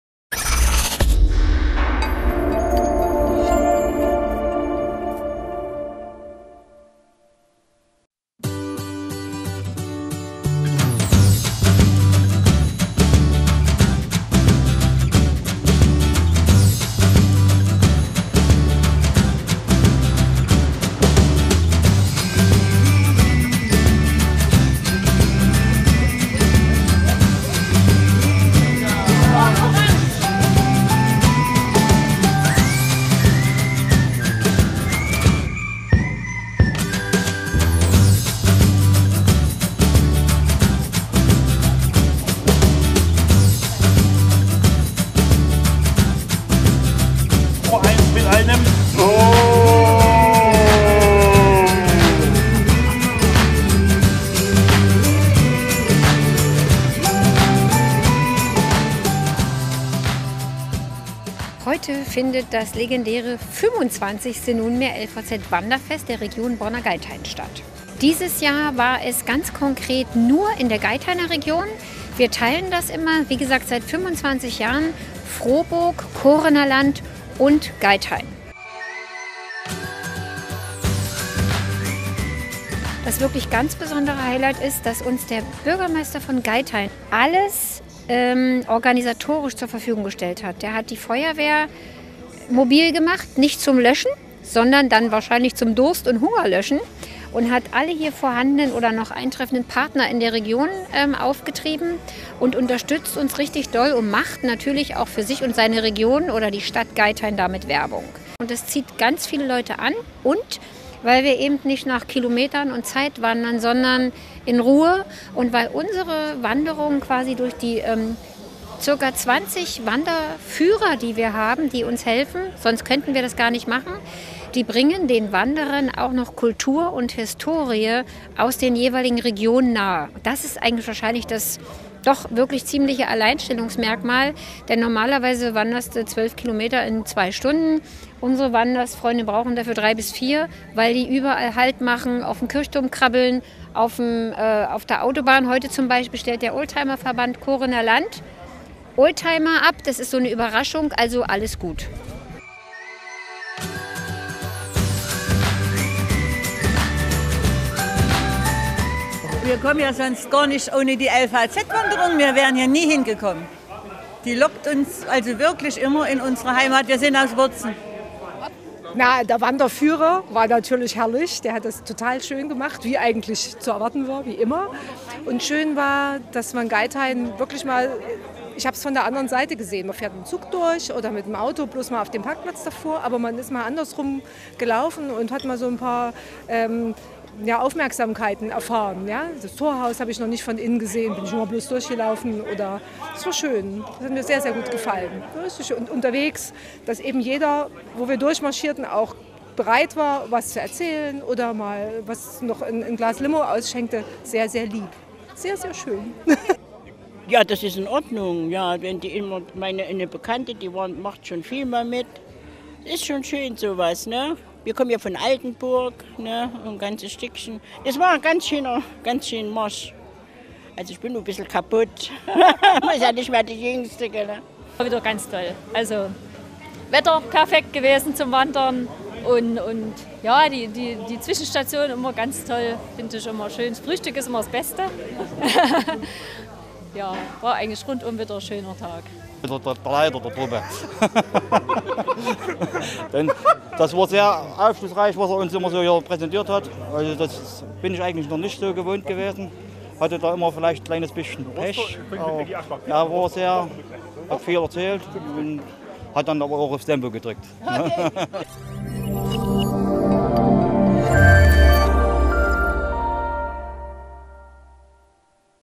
LVZ-Wanderung rund um Geithain 2018
Hier einige Impressionen von der LVZ-Wanderung rund um Geithain.